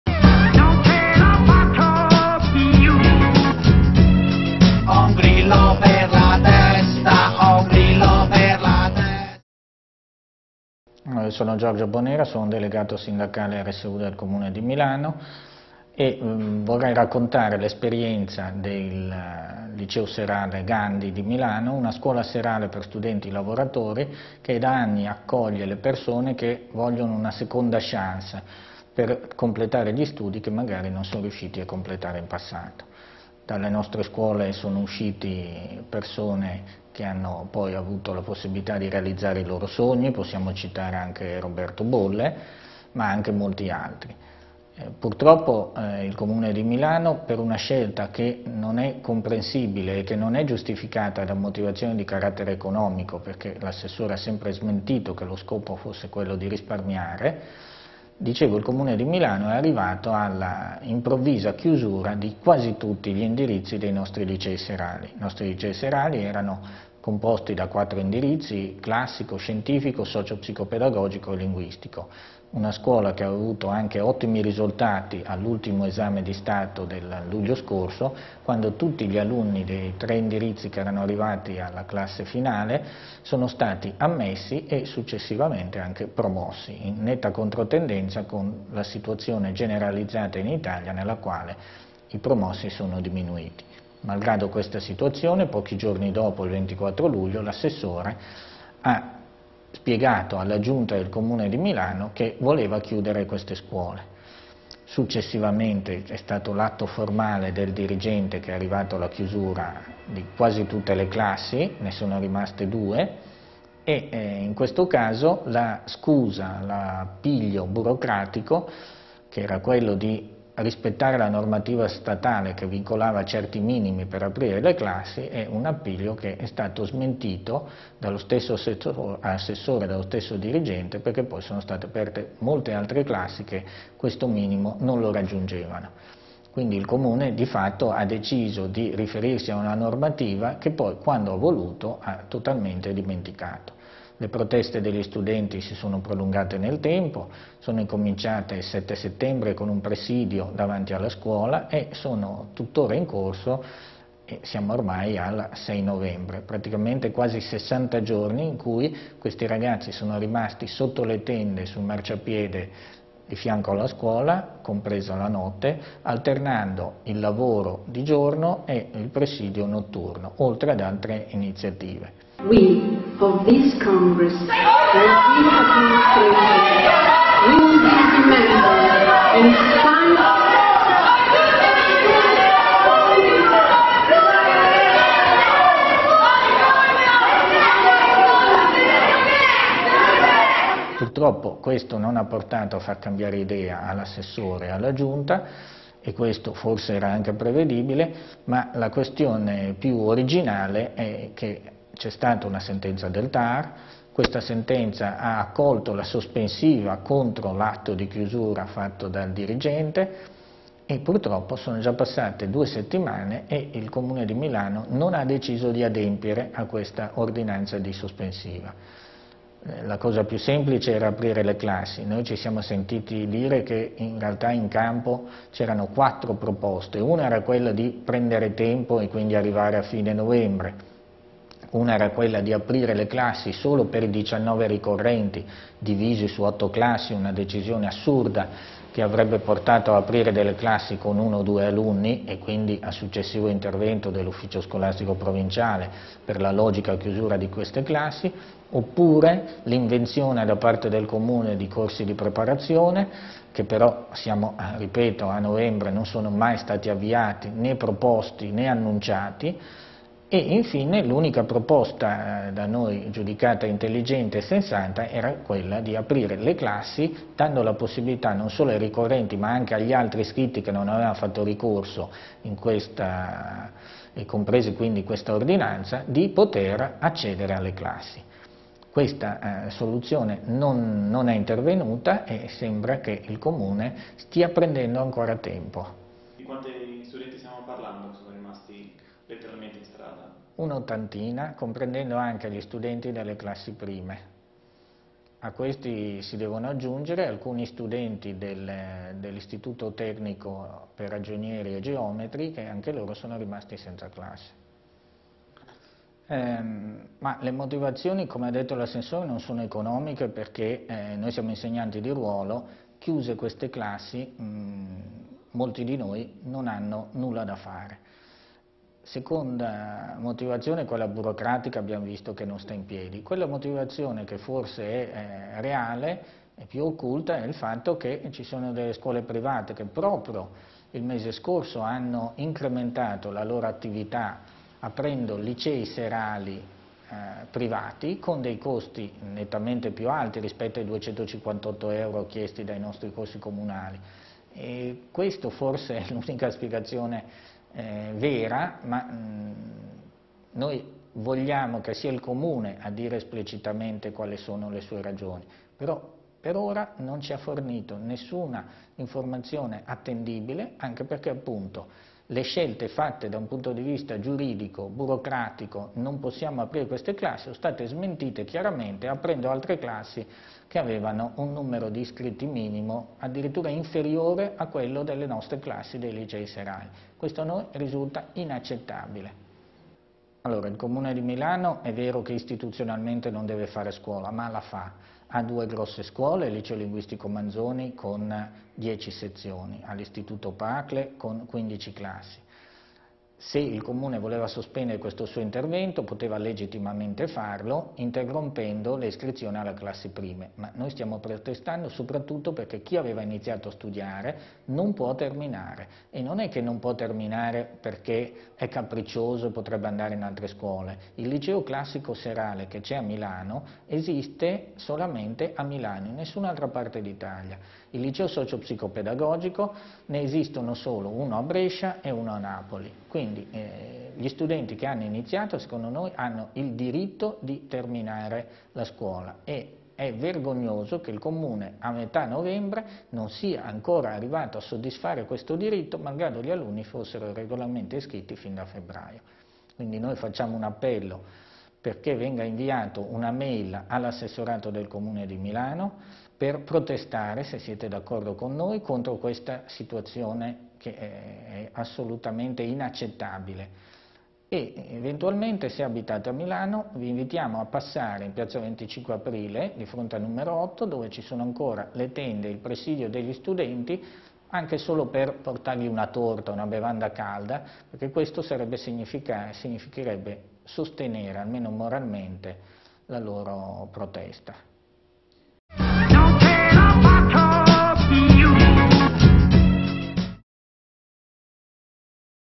Comune di Milano vs. Civico Liceo Gandhi
Il Comune di Milano decide arbitrariamente di chiudere il liceo Gandhi e lascia per strada decine di studenti - Tutti i libri e i DVD di Beppe Grillo sono disponibili su